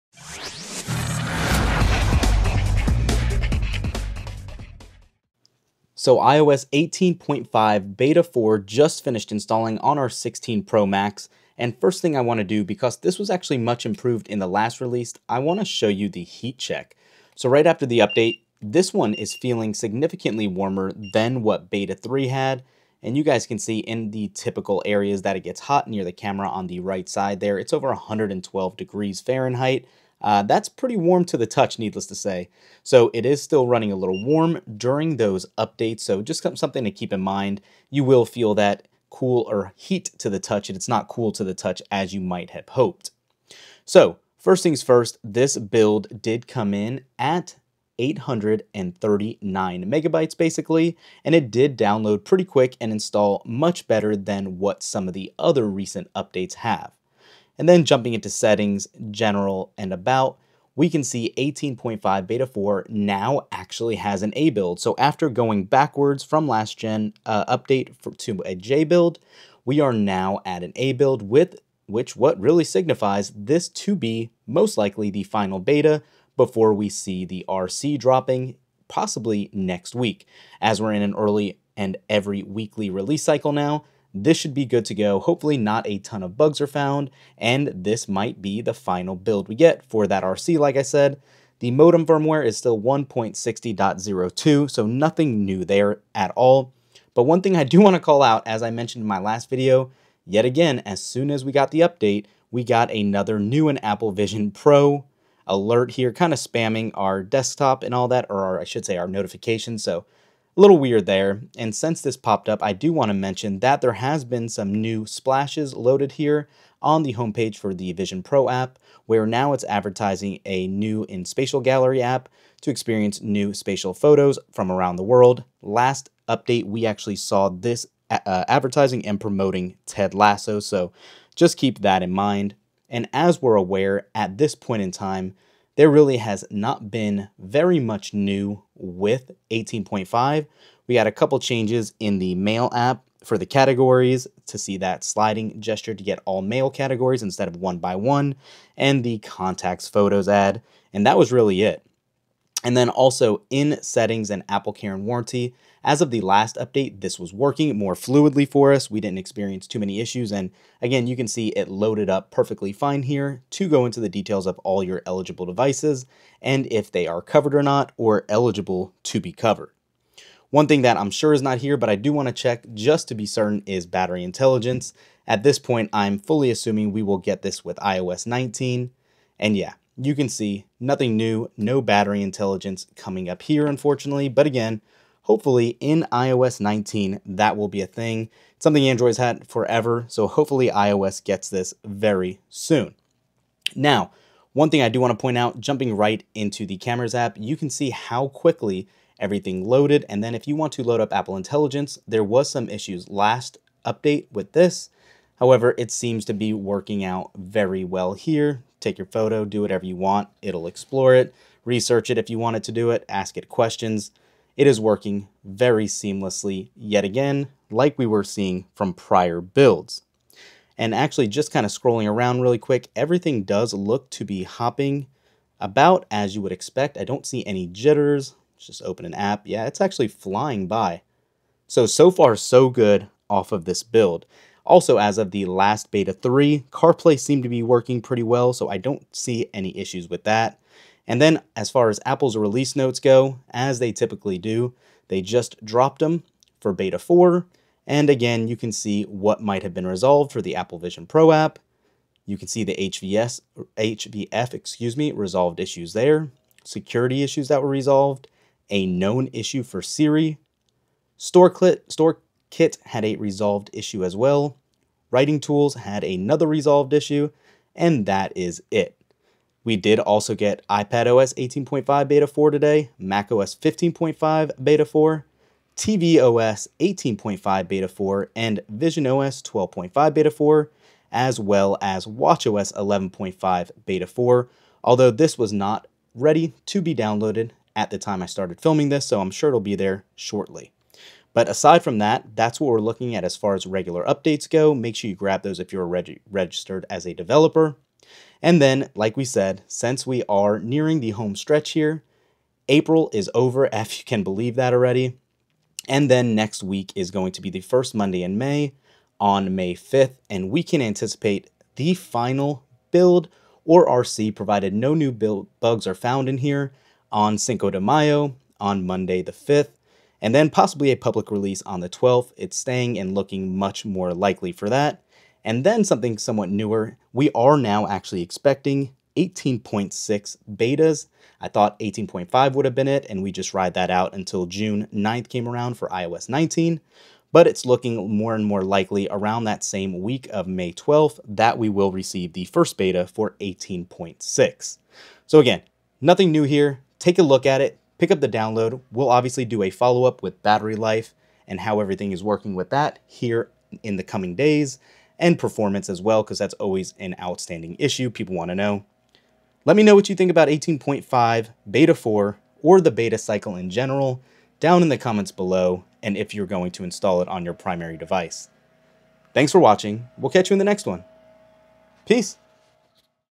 iOS 18.5 Beta 4 Is OUT- They Finally FIXED It!
After a rocky experience with iOS 18.5 betas, it really feels right of the bad that Apple fixed a lot of the bugs under the hood with beta 4. Lets take a quick look!

65W UGreen Bonus not in video